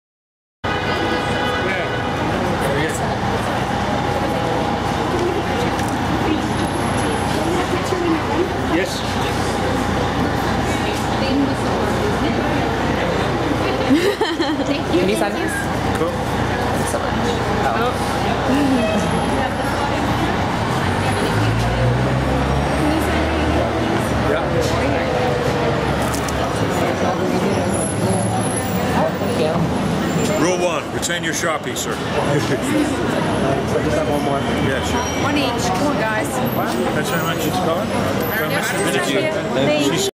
Mark Williams - Signing Autographs at his NYC Hotel
Mark Williams signed autographs at his hotel in New York City. For authentic autographed memorabilia of Mark Williams and many other celebrities, check out TopPix Autographs Get the Best Deals on Authentic Autographs on our eBay Store